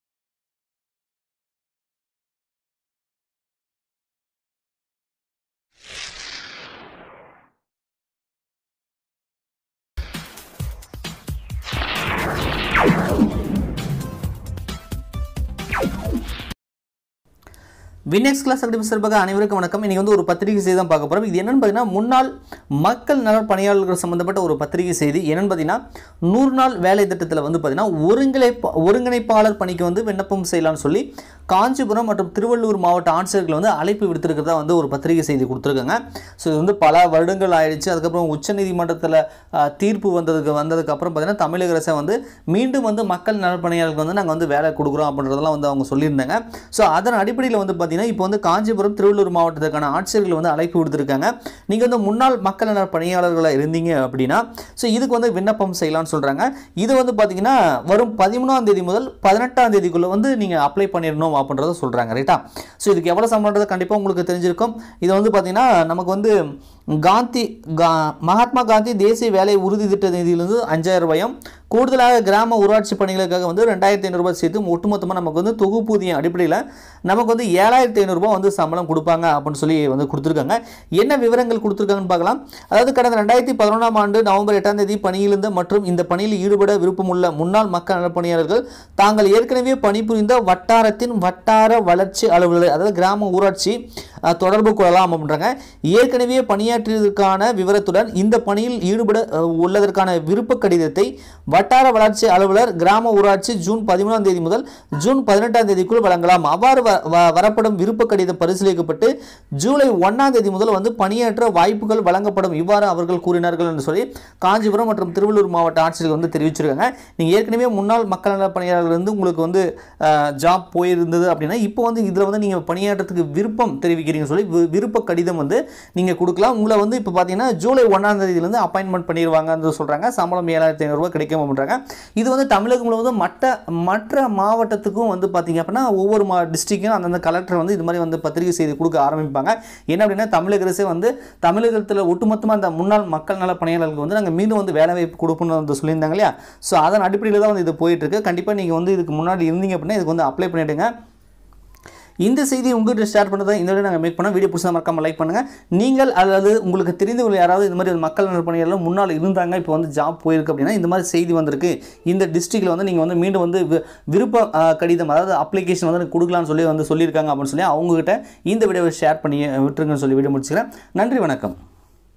ஒருங்கிணைப்பாளர் பணிக்கு விண்ணப்பிக்கலாம்/ சம்பளம் 7500/ Tamilnadu government jobs 2022
ஒருங்கிணைப்பாளர் பணிக்கு விண்ணப்பம் செய்ய காஞ்சிபுரம் மற்றும் திருவள்ளூர் மாவட்ட ஆட்சியர்கள் அழைப்பு விடுத்து உள்ளார்கள்.

முன்னாள் மக்கள் நலப் பணியாளர்கள் இதற்கு வரும் ஜூன் 13 முதல் 18 க்குள் விருப்பம் கடிதம் கொடுக்கலாம்